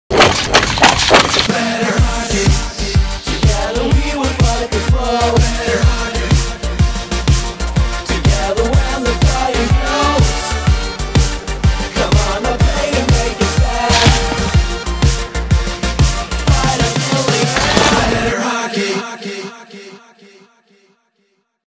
Extreme Hockey Flooring Tiles Shooting Kit XL | Better Hockey
Now you can train shooting at your own backyard. Extreme Hockey Flooring Tiles have amazing slide with regular pucks, The smart passer let you train passing and one-timers without training buddy. The goal has a built in backstop and shooting targets.